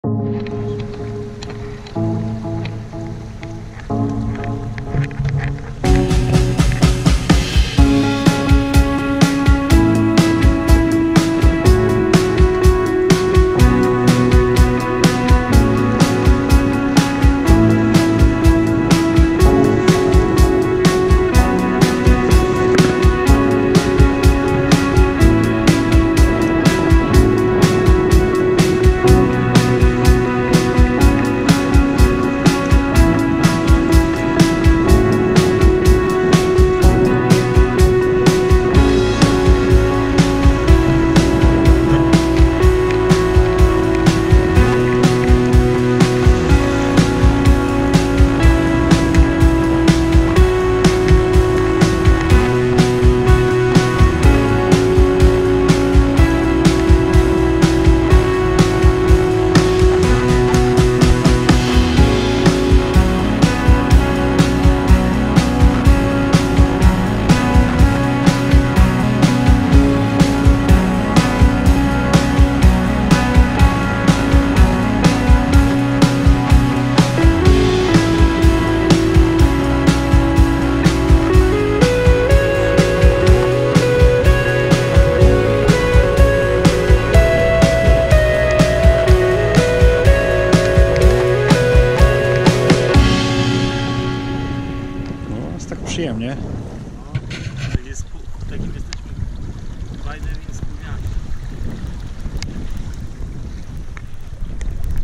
Sailing @ Mamry Lake, Nov 2019 _1
Windy Sunday 3rd of Nov spent in Northern part of Masurian Lakes area, in this case going back to the harbor through Mamry Lake on Saturn25 L Cruiser yacht. ...Smiling faces ;-)
Wietrzna niedziela 3-go listopada spędzona na północnych Mazurach, tutaj akurat w drodze do portu poprzez Jezioro Mamry, na jachcie Saturn25 L Cruiser. ...Banan na twarzach ;-)